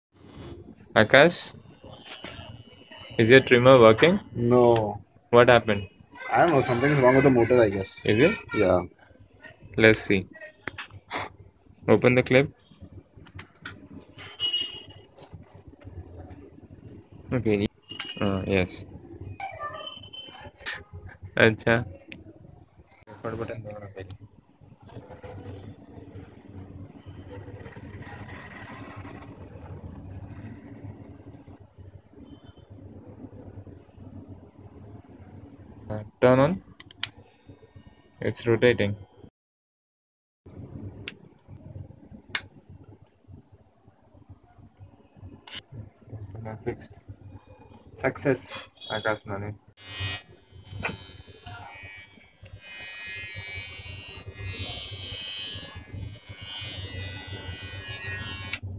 how to fix / repair Trimmer DIY
Trimmer stopped working after trying to cut plastic wrap from it.